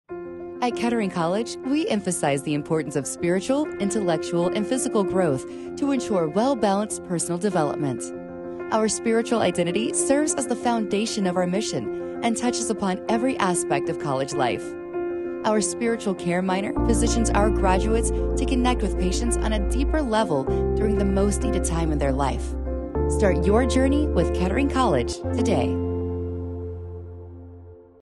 Kettering College: Faith-Based Campus with a Culture of Service
Kettering College believes in guiding our students to be their best, both personally and professionally. As a faith-based college, we walk alongside them to grow and explore their spiritual side. Our goal is not to convince anyone to follow a certain path, but rather to embrace a life of service and understand the importance of this. to learn more.